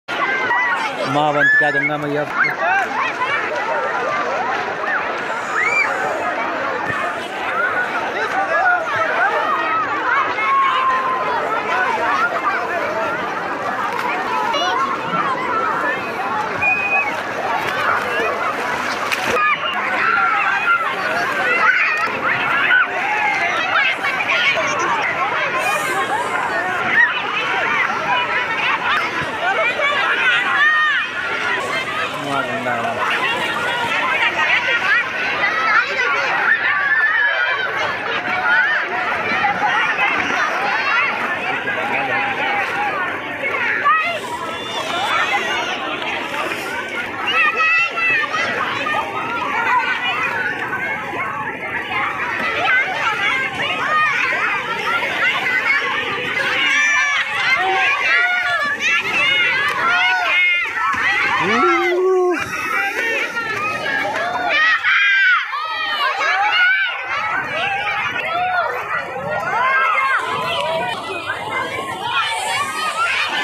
1 लाख से ज्यादा भक्तो ने किए गंगा मां के दर्शन | Ganga Maiya Darshan 2022🔥🔥ganga
गंगा स्नान के लिए उमड़ा जन सैलाब | Ganga snan Video🔥🔥🔥🔥